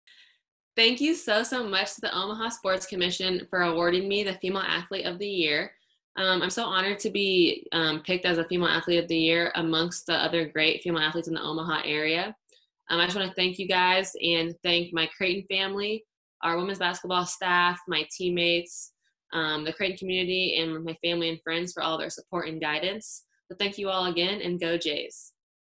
2020 OSCA Acceptance Speech - Jaylyn Agnew (Female Amateur Athlete of the Year)
Jaylyn Agnew accepts the 2019-20 Omaha Sports Commission Award as Female Amateur Athlete of the Year.